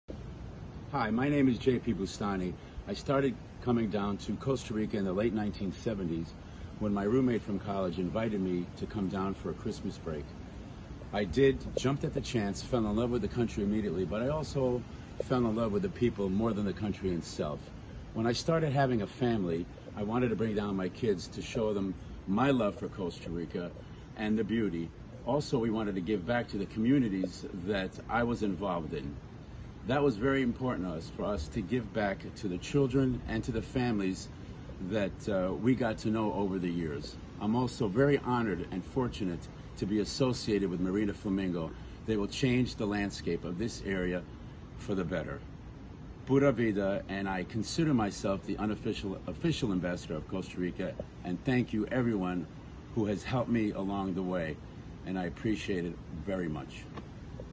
It's the people of Costa Rica
JP speaking about the people of Costa Rica.